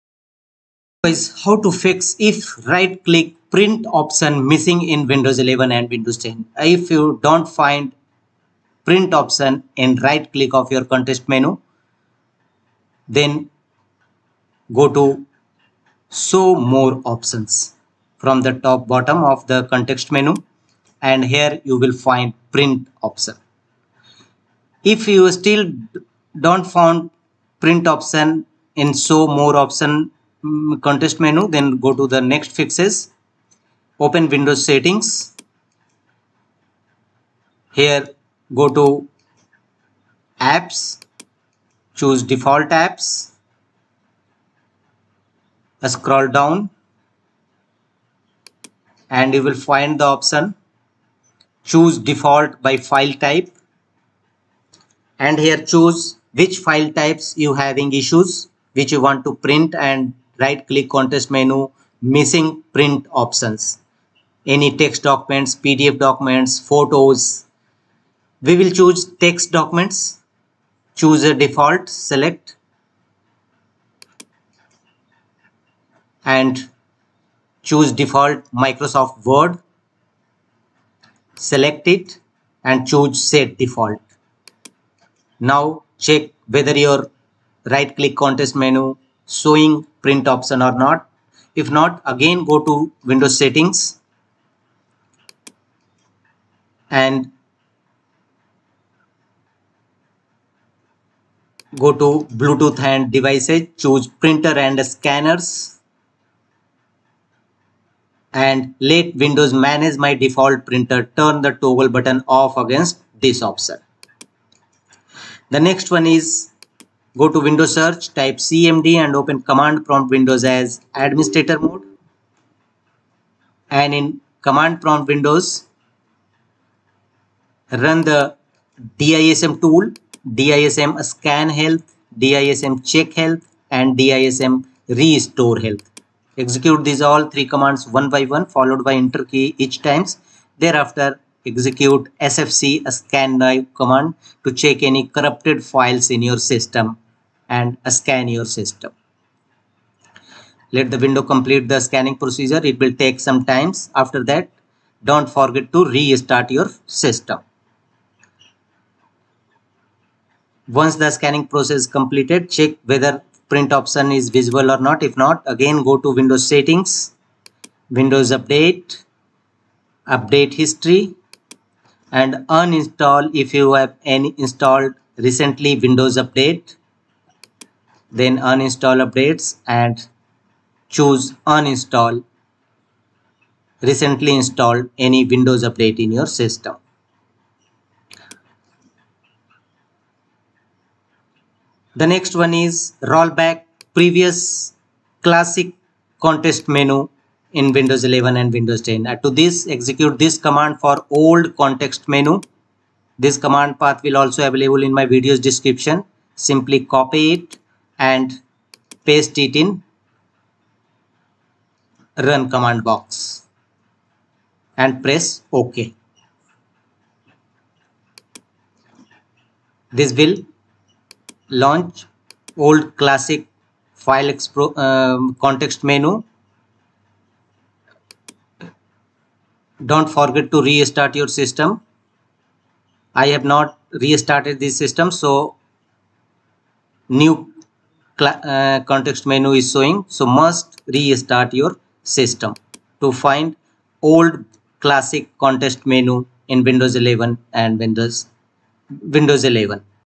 Print option in Right Click Context menu not showing in Windows 11 Fixed
How to Fix Print option in Right Click Context menu missing on Windows 11

Execute this command for Old context menu

reg.exe add "HKCU\Software\Classes\CLSID\{86ca1aa0-34aa-4e8b-a509-50c905bae2a2}\InprocServer32" /f /ve

Execute this command for New context menu

reg.exe delete "HKCU\Software\Classes\CLSID\{86ca1aa0-34aa-4e8b-a509-50c905bae2a2}" /f